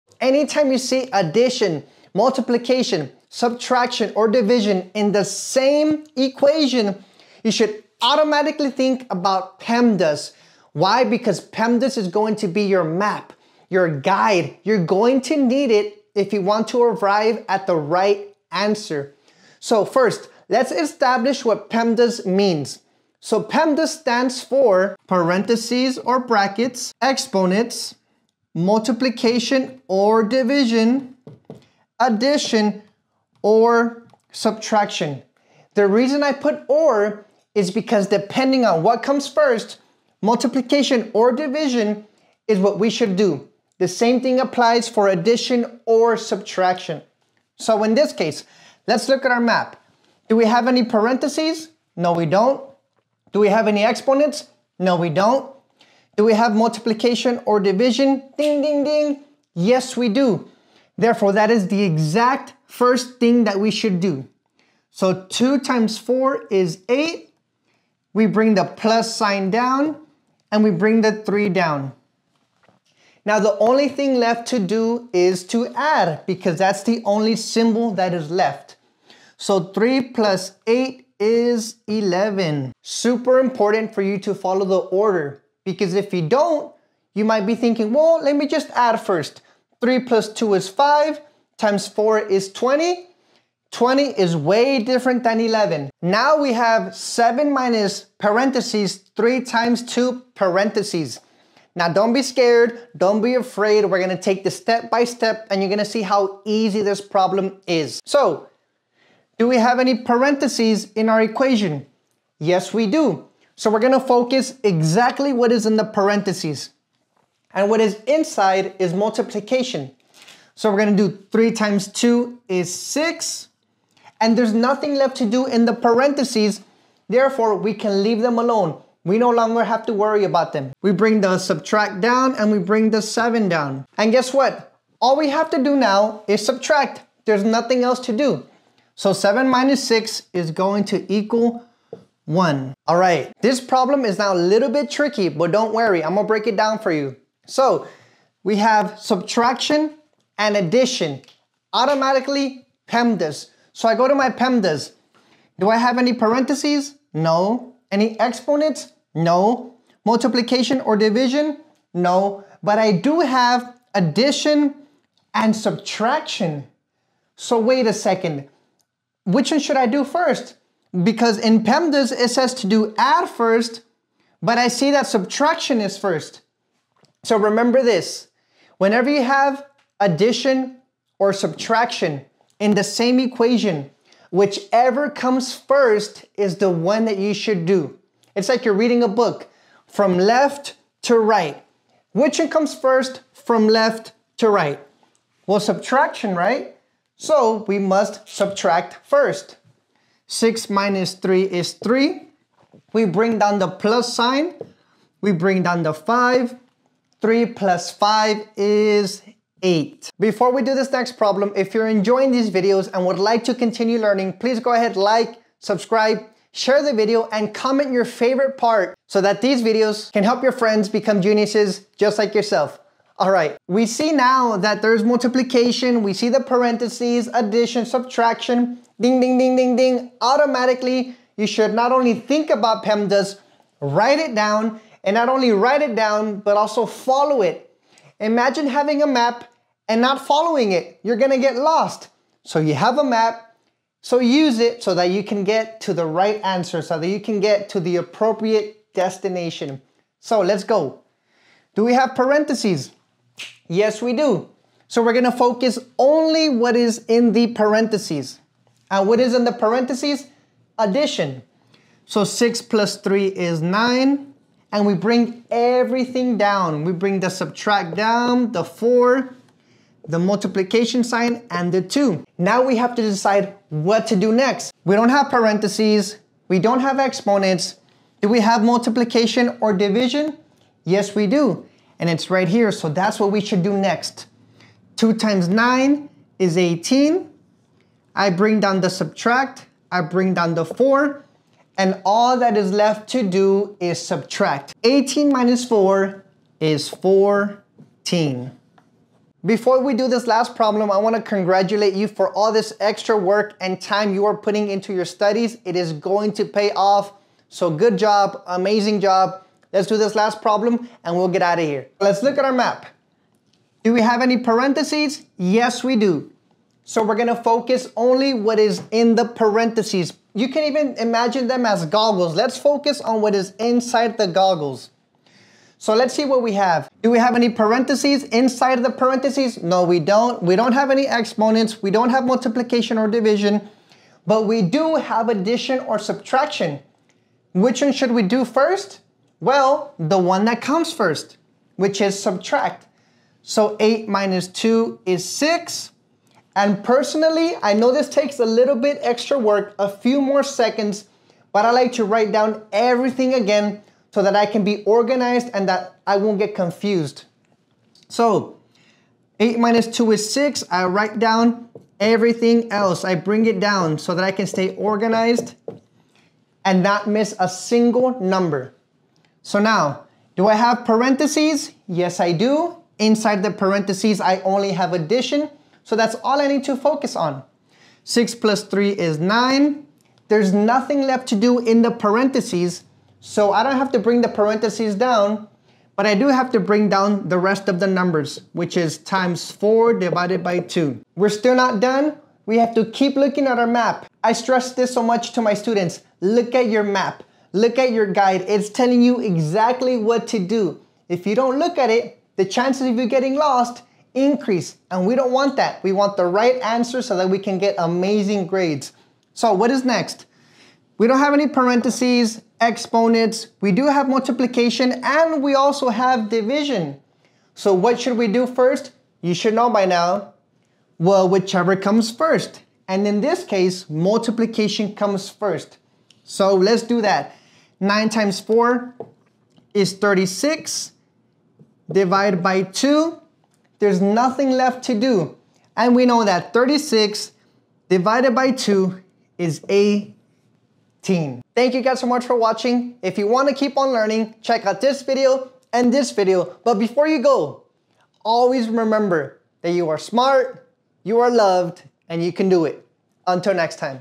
Order of Operations Made Easy! (PEMDAS)
Struggling with order of operations? In this video, we break down PEMDAS in a simple and easy-to-understand way. Learn how to solve math problems with confidence!

Timestamps

0:00 PEMDAS Problem 1 (Order of Operations)
1:36 PEMDAS Problem 2 (Order of Operations)
2:22 PEMDAS Problem 3 (Order of Operations)
3:30 PEMDAS Problem 4 (Order of Operations)
4:59 PEMDAS Problem 5 (Order of Operations)
7:21 You are smart. You are loved. You can do it.